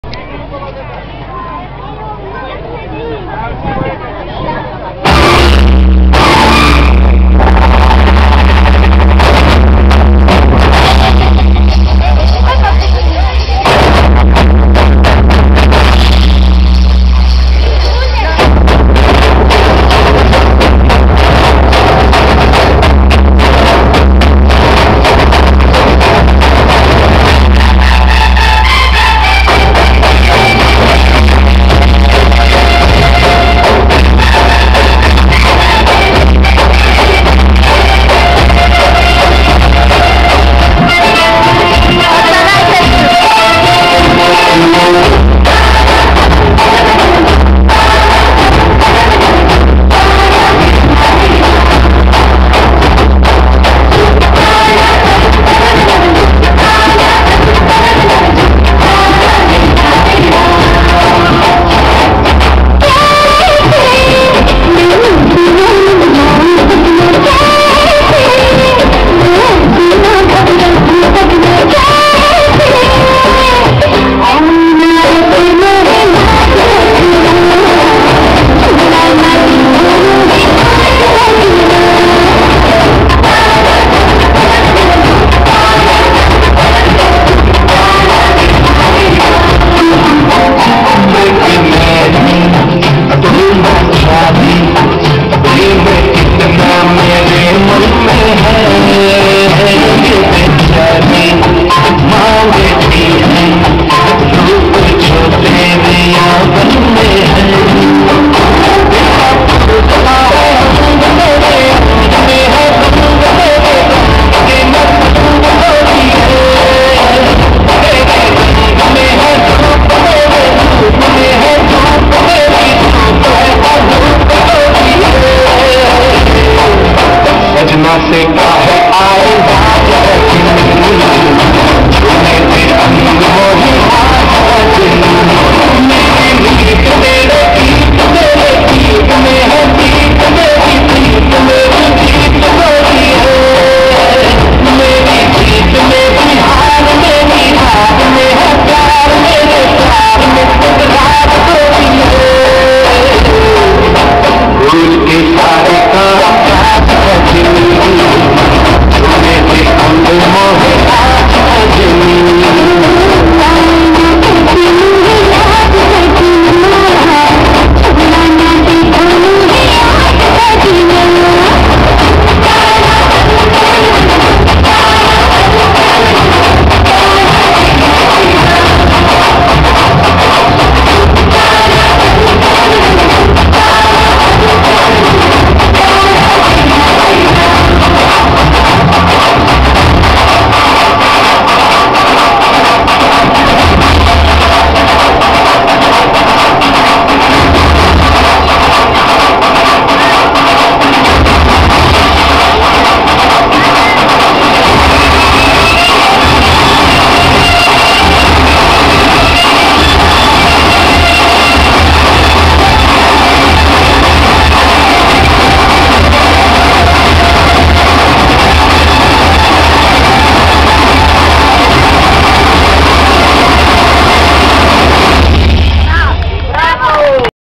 ,,STEP BY STEP-PAULESTI,, DANS INDIAN 25-05-2013
DANS PREZENTAT LA ,, SARBATOAREA CIRESELOR,, COM PAULESTI . PENTRU MAI MULTE INFORMATII VIZITATI SI PAGINA TRUPEI LA